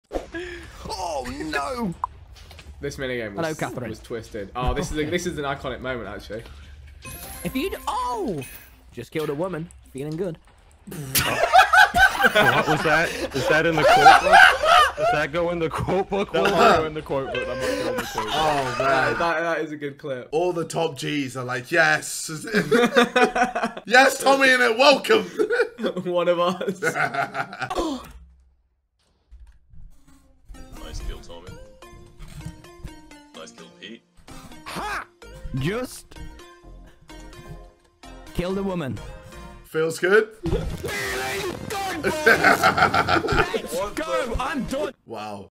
KSI Reacts to TommyInnit "Just Killed a Woman Feeling Good!"
KSI and Vikkstar react to tommyinnit just killed a woman feeling good funny moment